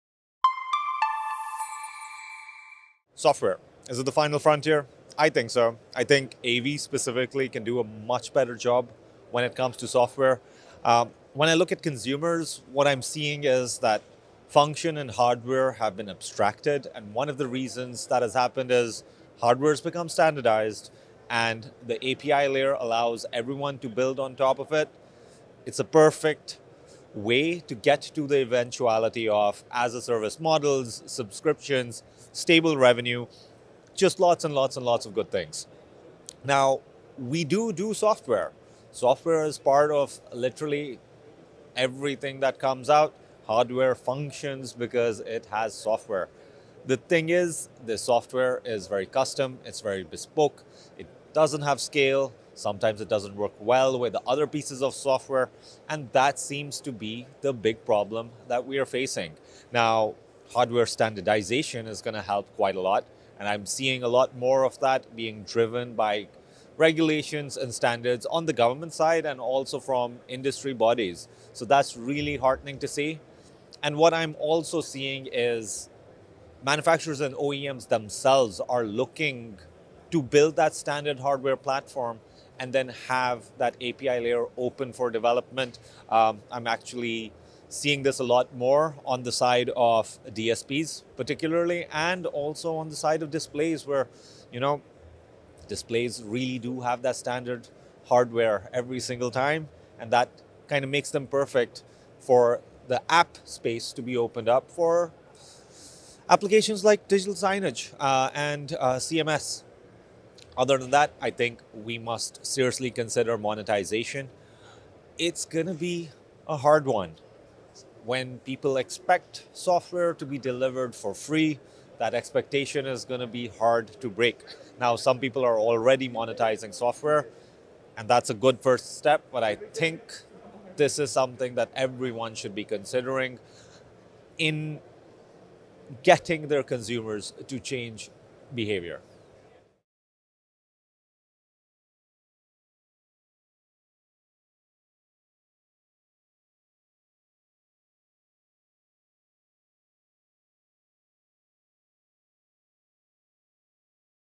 Software's role in AV transformation | What's Next? | ISE 2025
In this What's Next? video, Hurrairah bin Sohail delves into the crucial role of software in transforming AV technology. Discussing custom solutions, hardware standardisation, and the future of monetizing software, they highlight key trends shaping the industry.